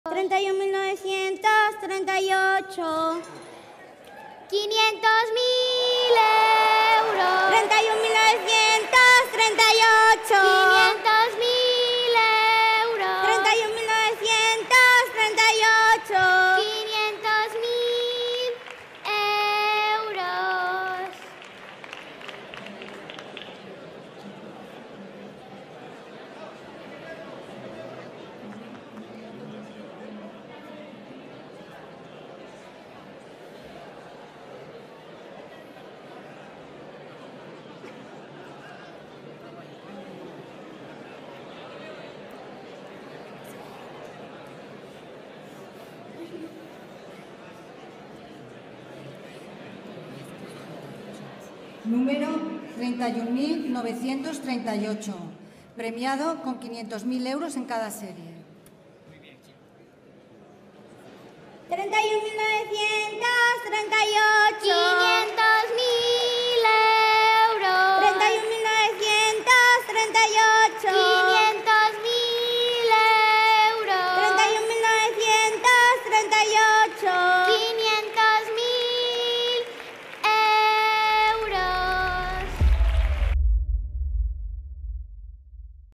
Sale el tercer premio de la Lotería de Navidad: el 31938
El tercer premio, el 31938, se ha vendido exclusivamente en la población de Biar (Alicante). Cada décimo es agraciado con 50.000€.